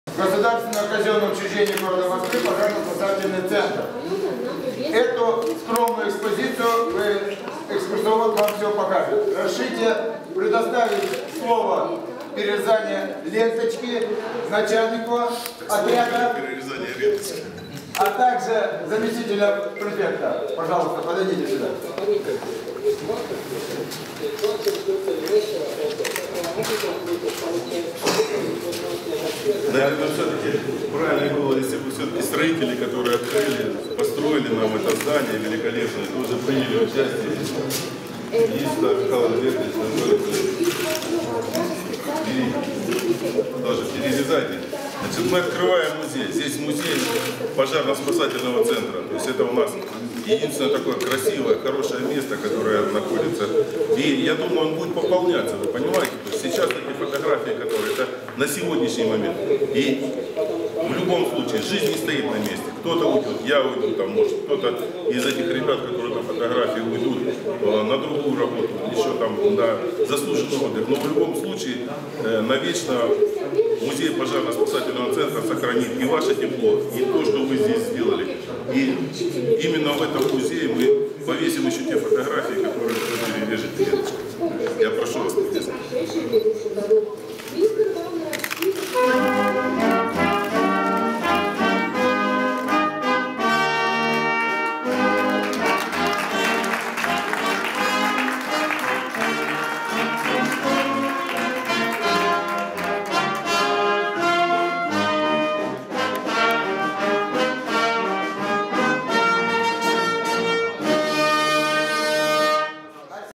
Открытие музея в новом пожарном депо в Алабушево
27 декабря 2013 года, Зеленоград, технико-внедренческая зона «Алабушево». Видео Павла Чукаева, Подробности